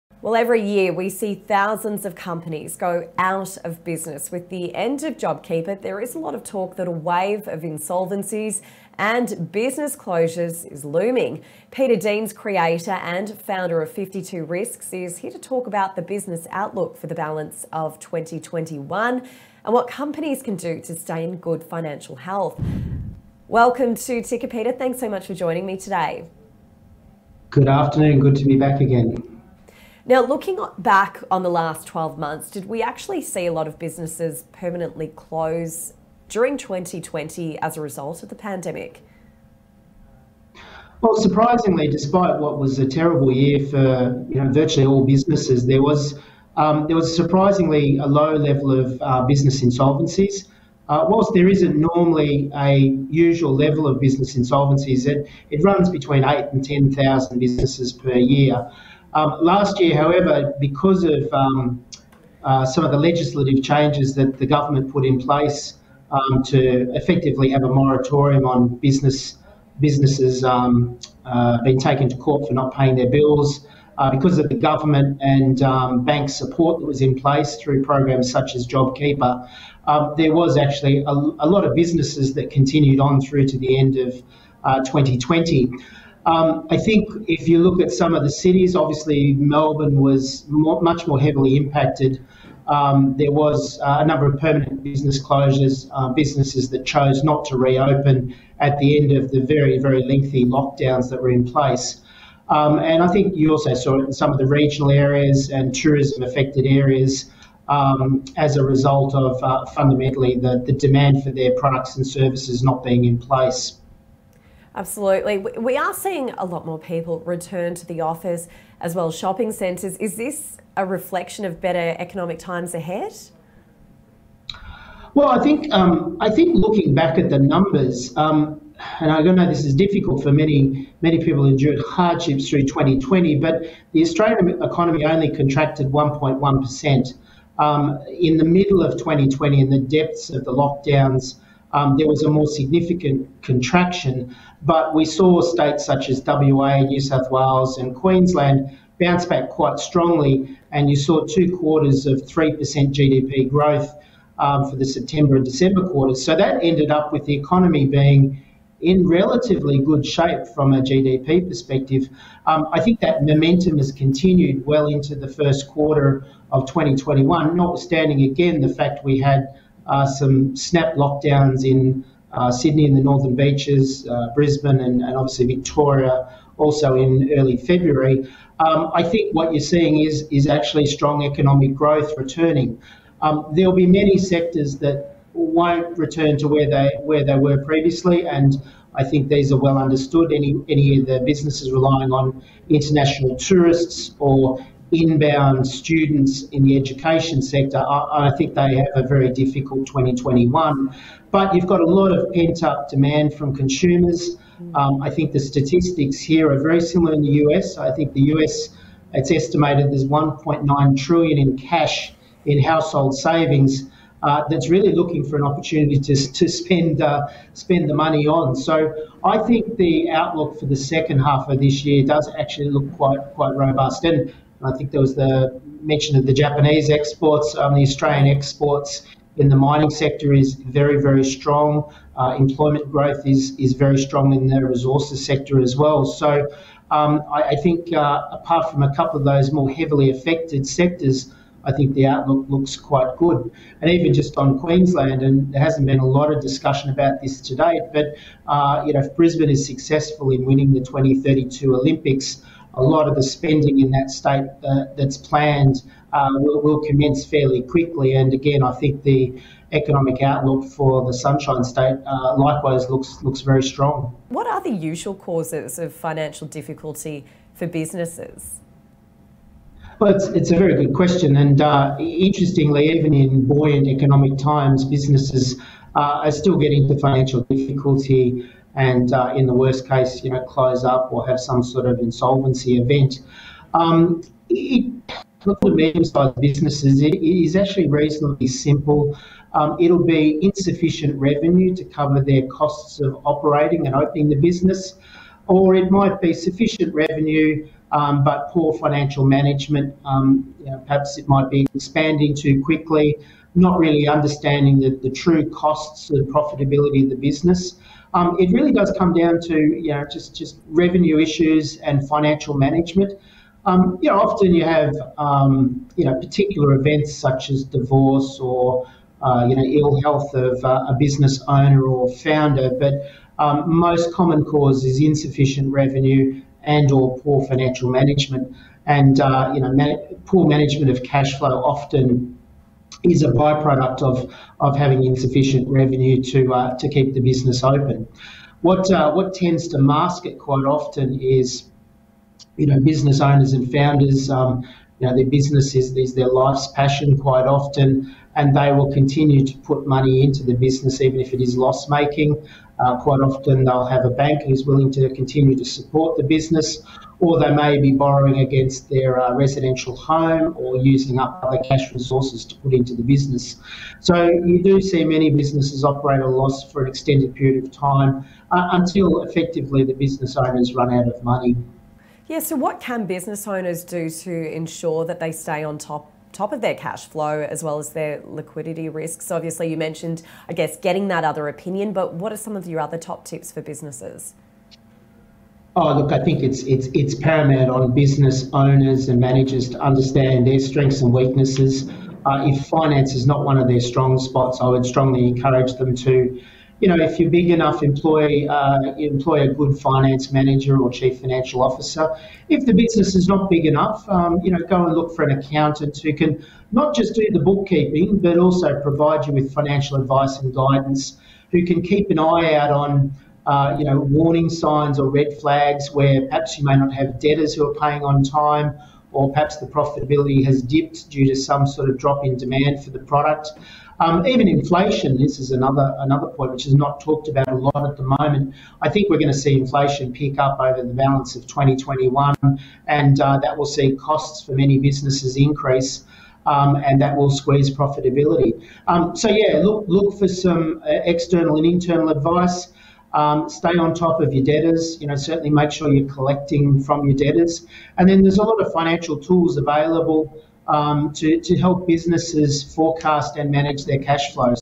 2021 Business Outlook with Peter Deans
💡📺 With the end of job keeper looming, there is a lot of talk of a wave of insolvencies and business closures ahead. Check out our mate Peter Deans who popped up on ticker NEWS with excellent insight on the business outlook for the balance of 2021 & what companies can do to stay in good financial health!